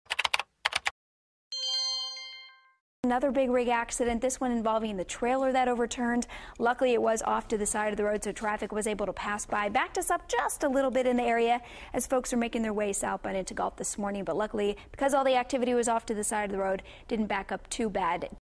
Big Rig Trailer Flips Along I-5
The trailer of a big rig flipped on its side Monday morning along Interstate 5 near Twin Cities Road in Sacramento County.